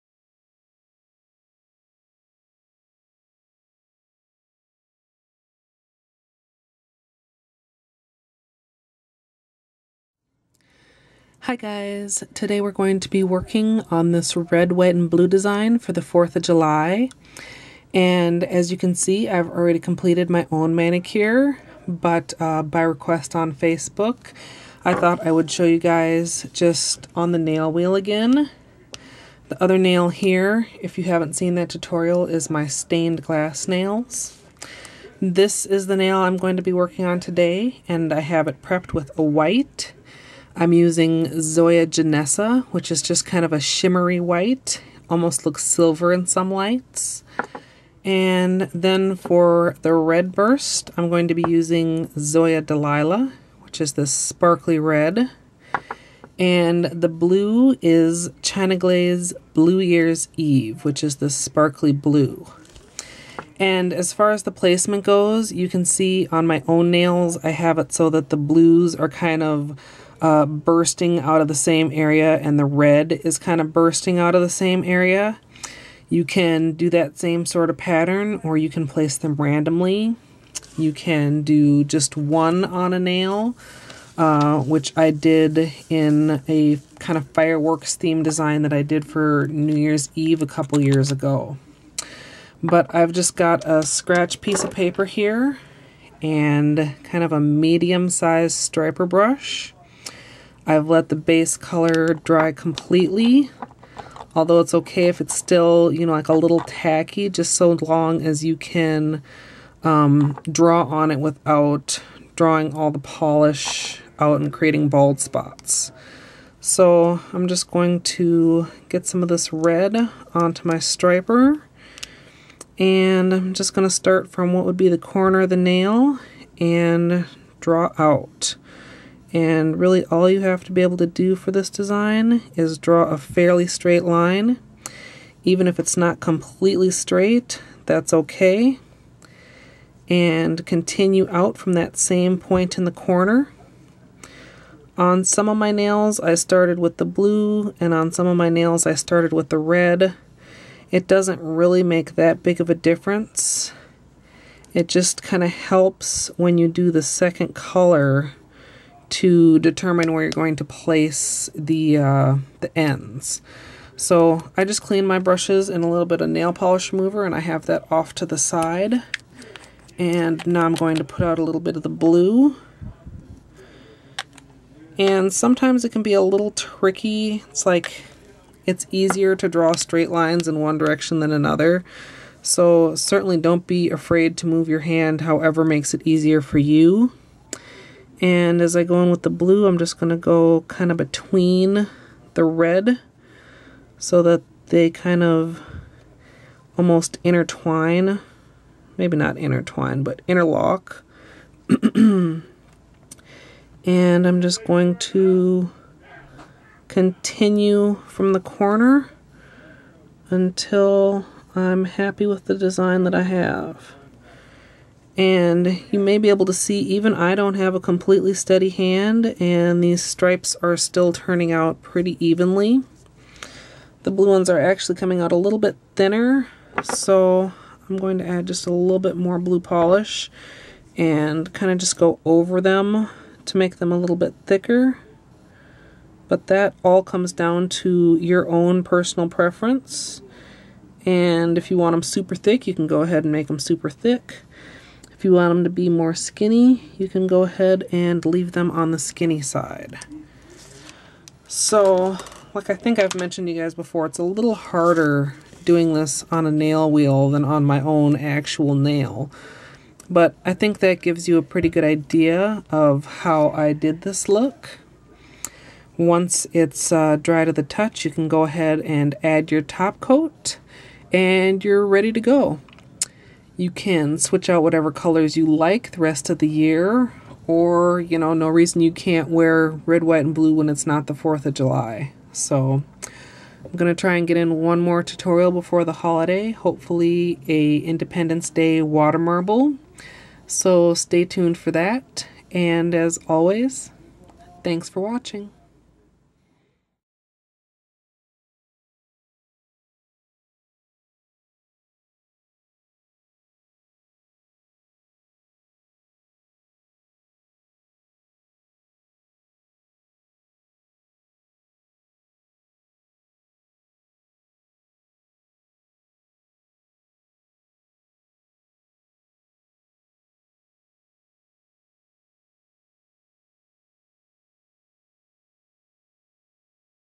Red White & Blue Burst for 4th of July | DIY Nail Art Tutorial
Today I'll be showing you how to do this patriotic fireworks inspired design.  ▼All products used linked below!▼

I was going to call this a fireworks design, but with the white background it didn't seem suitable since fireworks are normally at night =)  So they're just colorful bursts LOL.

-Base coat (of your choice)
-Zoya - Ginessa
-Zoya - Delilah
-China Glaze - Blue Yea'rs Eve
-Seche Vite Fast Dry Top Coat
-Nail Art Striper Brush

►Step-by-step◄
-Start out with clean, dry nails and apply a base coat
-Use Ginessa as your base color
-Create "bursts" of color coming from the corners of your naisl with Delilah and Blue Year's Eve
-Finish with a top coat!